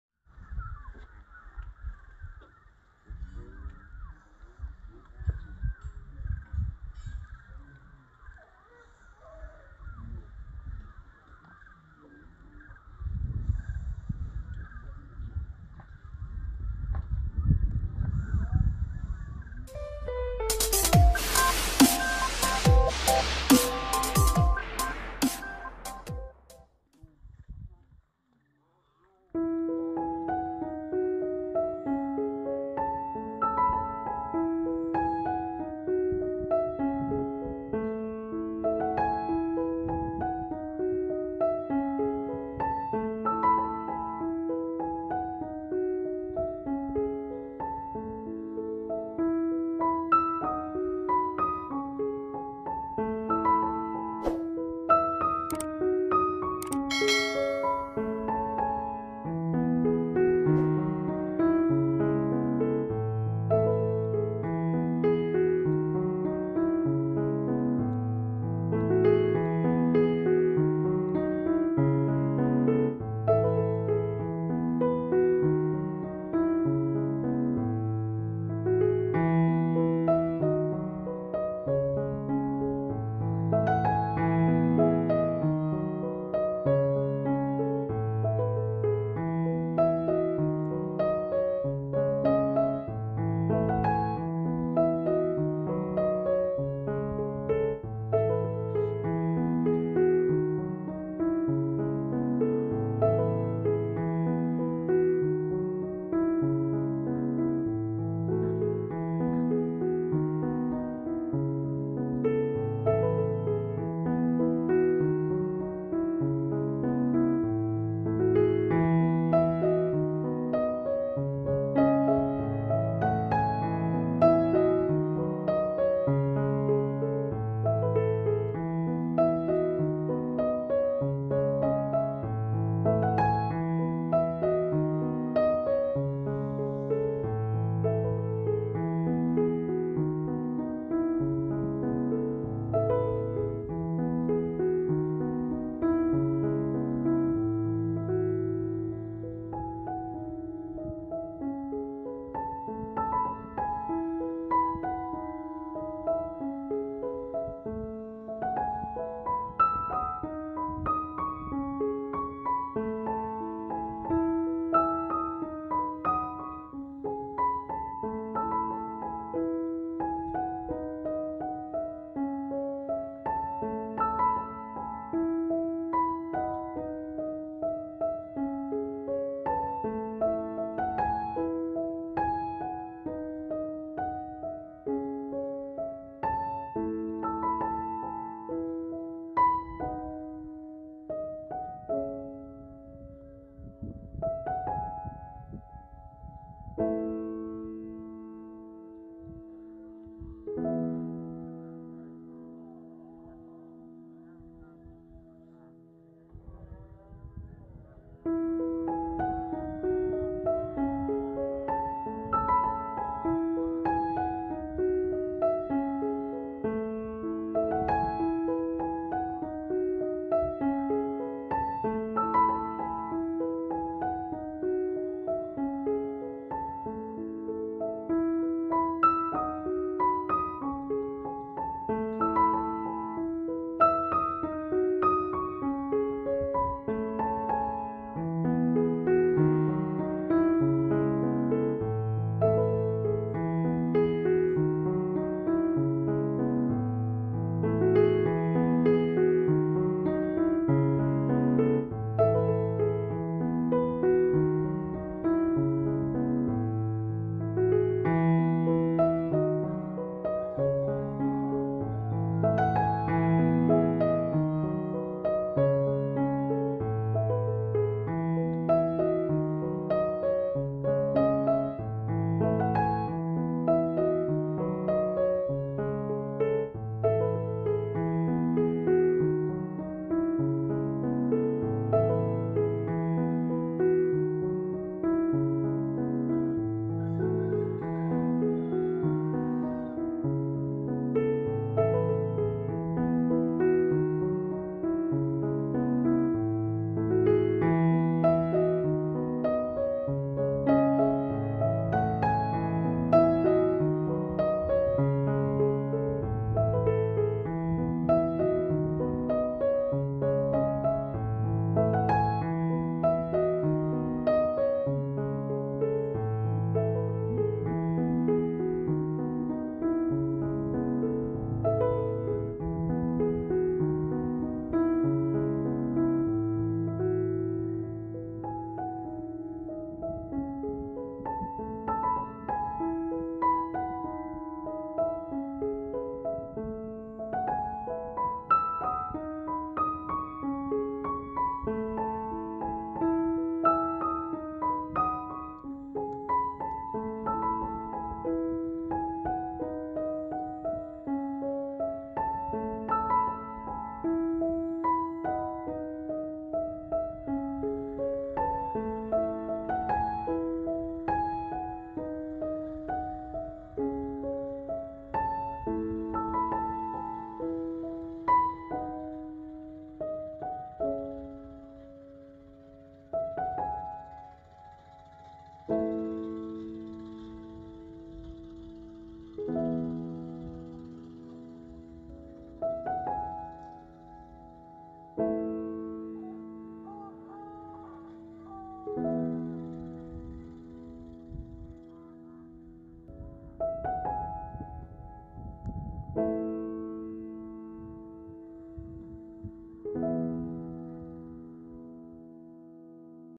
Indahnya Matahari Terbit || Lereng gunung sumbing
Indahnya Matahari Terbit dilereng gunung sumbing. keindahan yang jarang dilihat orang banyak. letaknya di kaliangkrik magelang